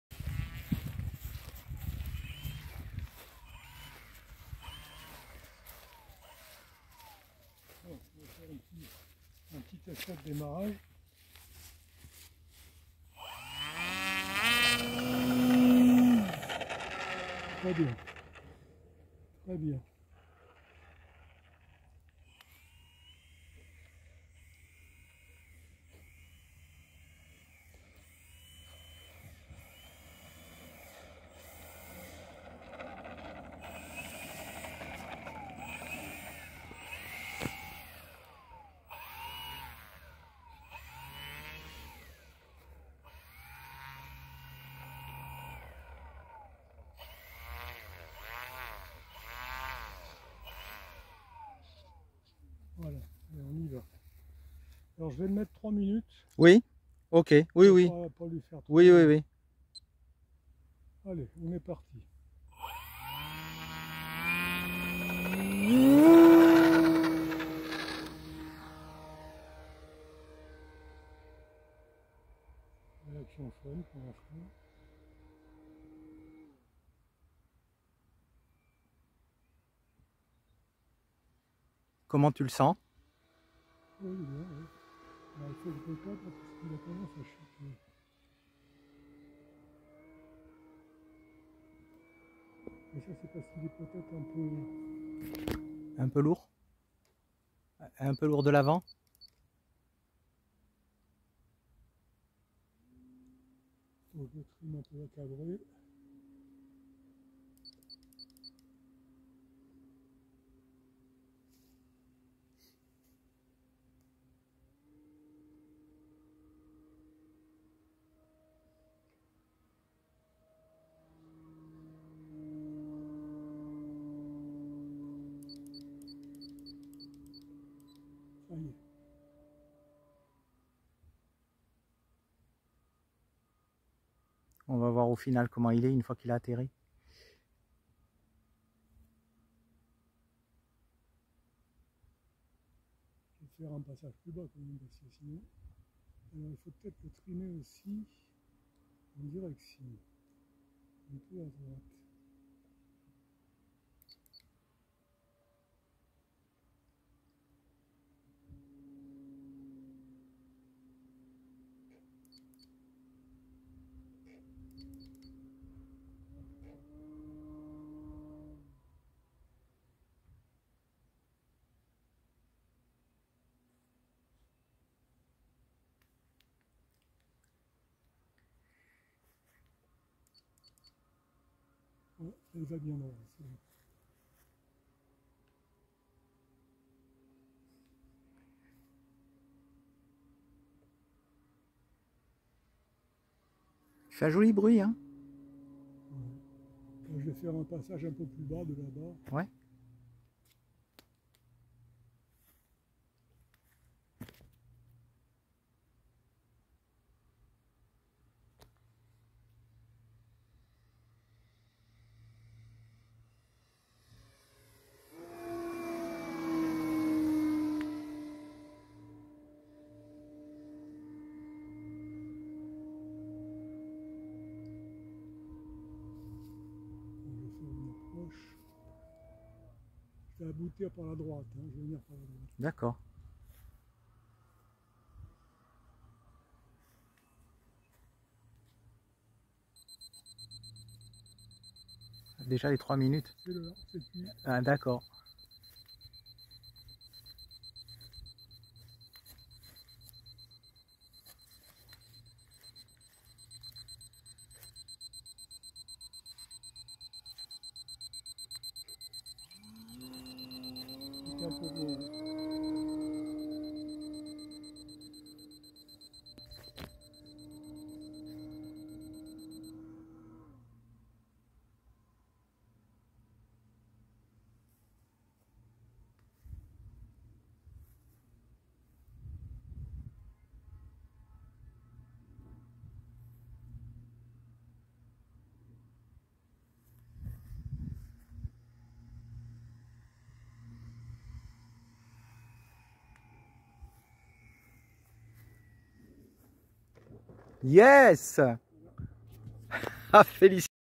Calmato 1erVol 28 10 2022
Premier vol du Calmato avec Michel Cavan aux commandes